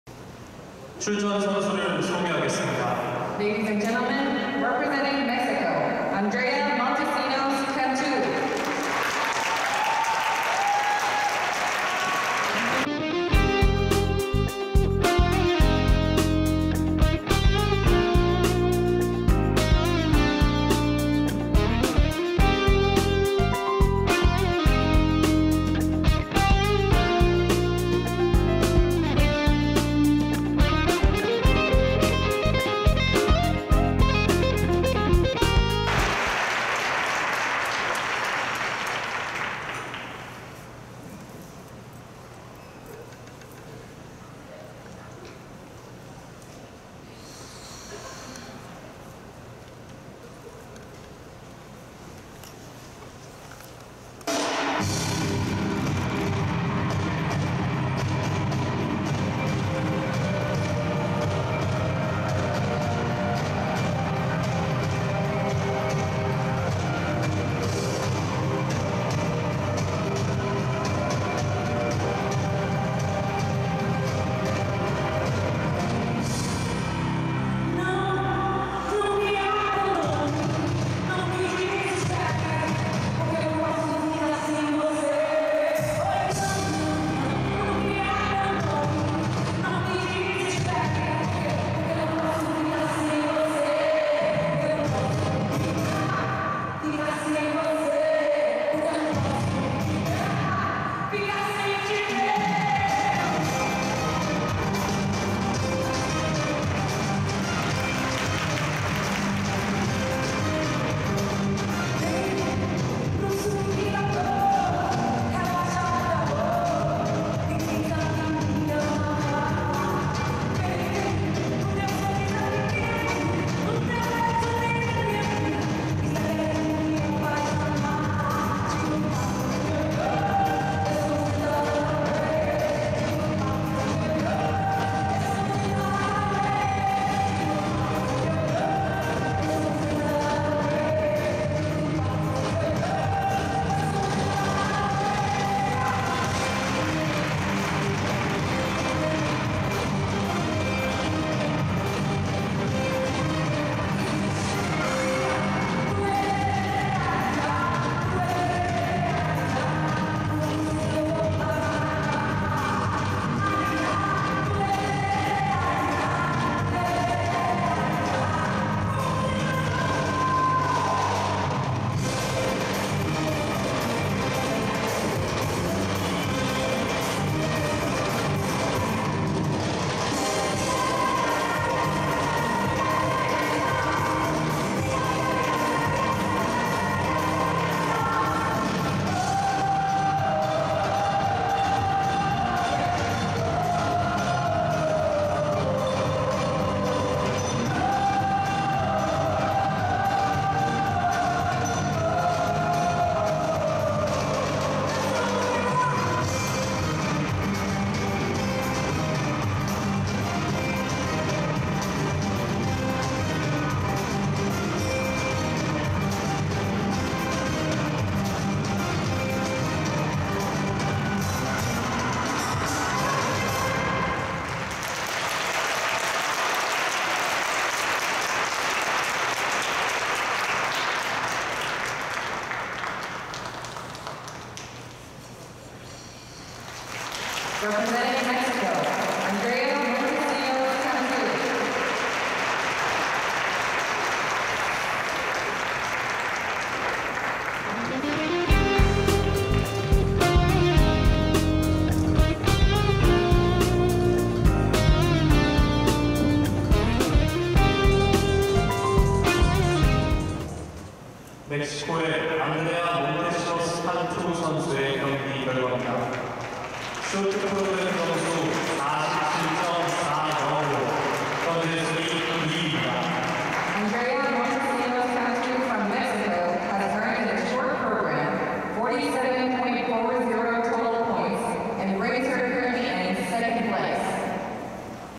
Andrea Montesinos Cantú (Mexico) - SP, 4CC 2020 Seoul
4 Continents Figure Skating Championship 2020 Seoul Korea
Andrea Montesinos Cantú (Mexico) - Ladies,  Short Program
Score : 47.40 Rank 15 (Total Rank 16)

四大陸フィギュアスケート選手権
四大洲 花样滑冰 锦标赛, 韩国 首尔
사대륙 피겨스케이팅 선수권대회